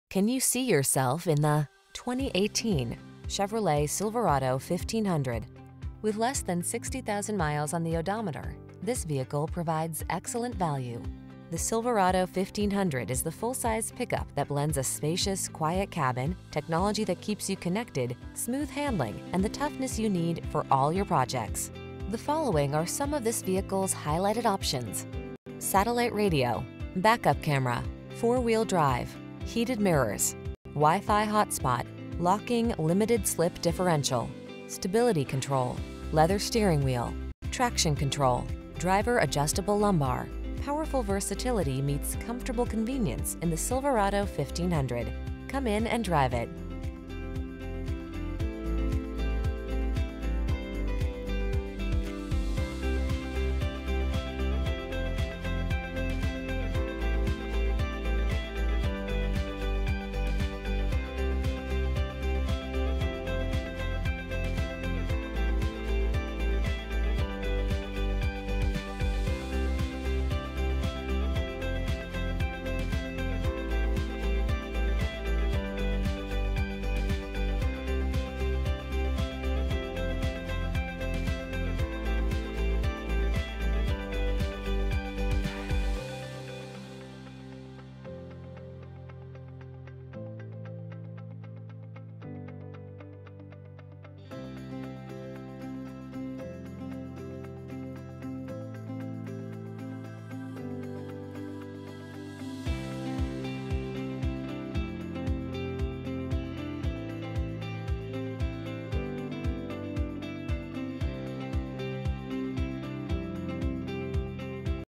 2018 Chevrolet Silverado_1500 Worthington, Columbus, Westerville, Powell, Delaware PXT101745A
Silver Ice Metallic Used 2018 Chevrolet Silverado_1500 available in Worthington, Ohio at Mark Wahlberg Chevrolet of Worthington. Servicing the Westerville, Powell, Delaware area.

700 E. Dublin-Granville Road

Clean CARFAX.LIFTED Custom Wheels 6-Speed Automatic Electronic with Overdrive 4WD Jet Black Cloth.Silver Ice Metallic 2018 Chevrolet Silverado 1500 LT LIFTED 4WD 6-Speed Automatic Electronic with Overdrive V8 16/22 City/Highway MPGOdometer is 14728 miles below market average!